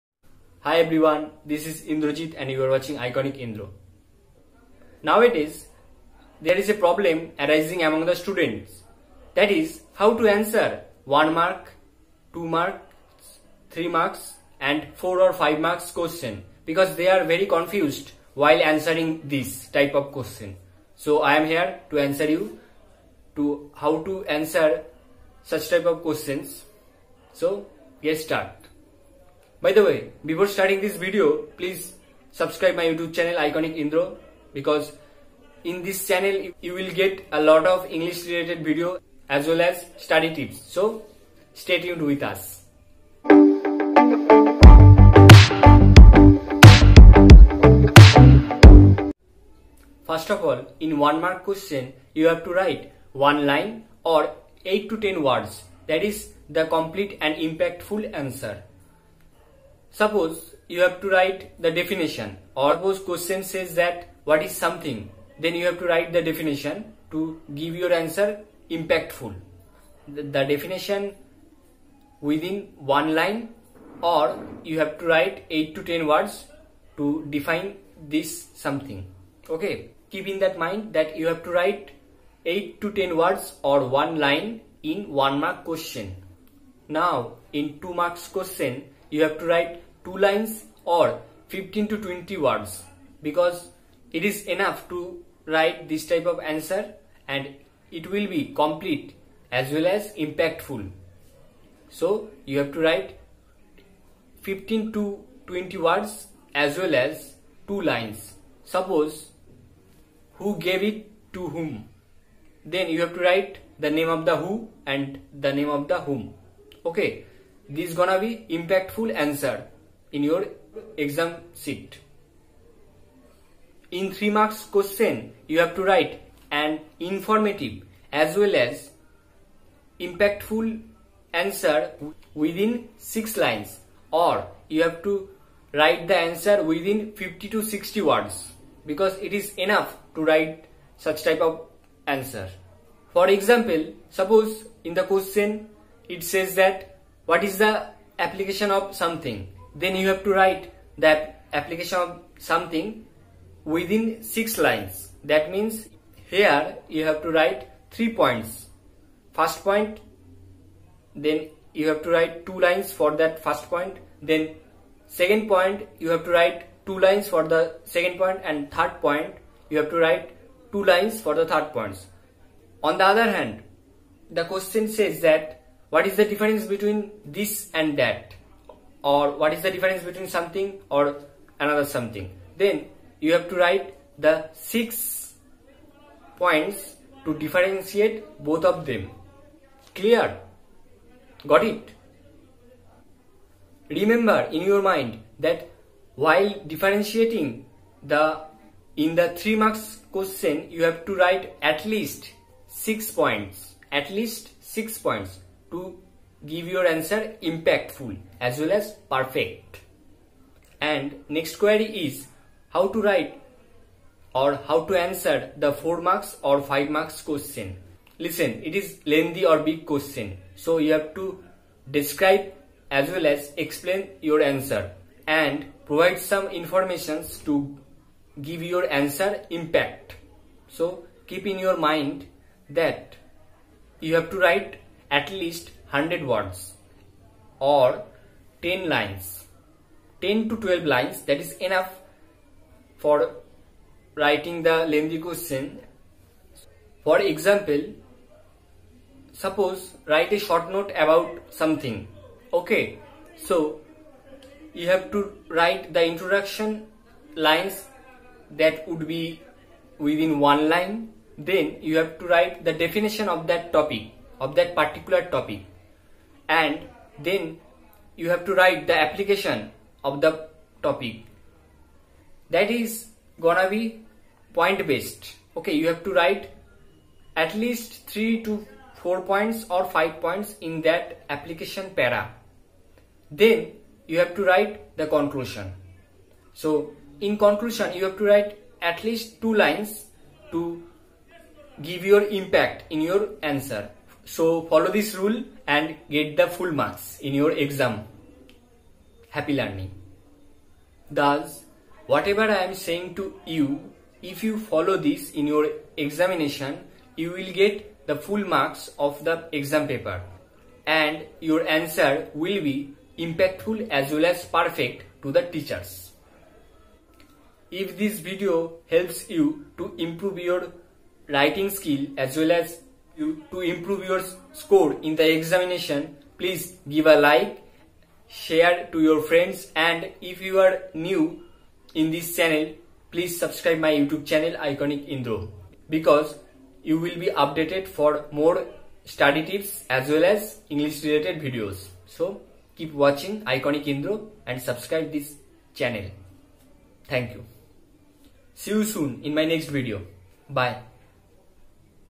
How To Answer 1, 2, 3 and 4 or 5 marks Questions
There is a major problem arising among the students about how to answer 1 mark, 2 marks, 3 marks and 4 or 5 marks questions. That's why in this video I am going to tell you about how to answer those different types of questions in a perfect way and how to score good marks in exam.

Happy learning :)

👉 Best English books to increase your vocabularies :-

• User ID - iconicindra